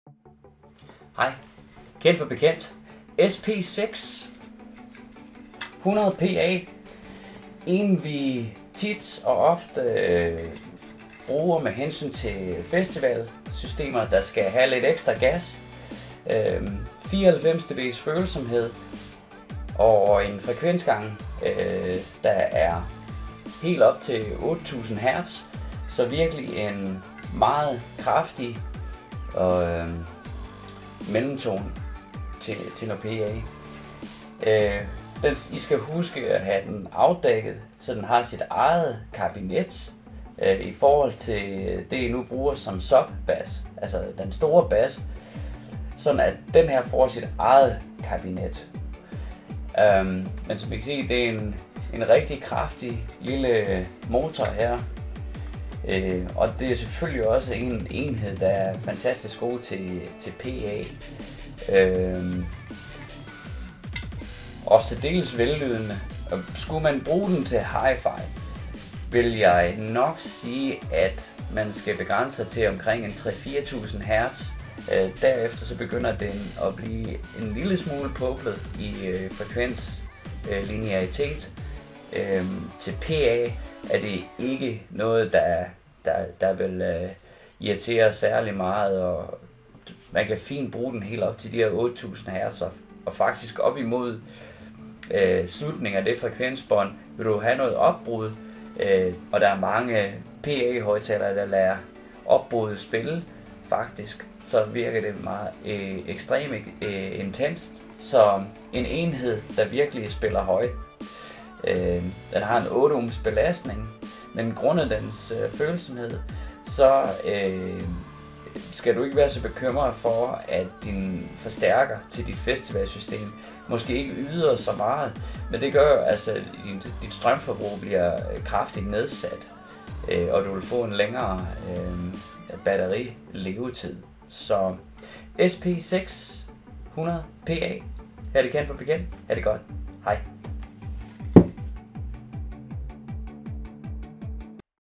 SP6-100PA PA mellemtone
SP6-100PA er hvad man vil kalde en PA mellemtone, det vil sige den spiller højt i mellemtone området og er super hårdfør. Det gør den gør den oplagt til professionelt brug, og vi har brugt den rigtigt meget i forbindelse med festivals anlæg.
Højtalere design hjælp i masse vis, og vi elsker og lege med det, og er med på et hvert niveau.
Den er specielt god hvis du lige er begyndt med højtaler byg, eller gerne vil havde lidt teoretisk kendskab, uden at det skal blive for tungt.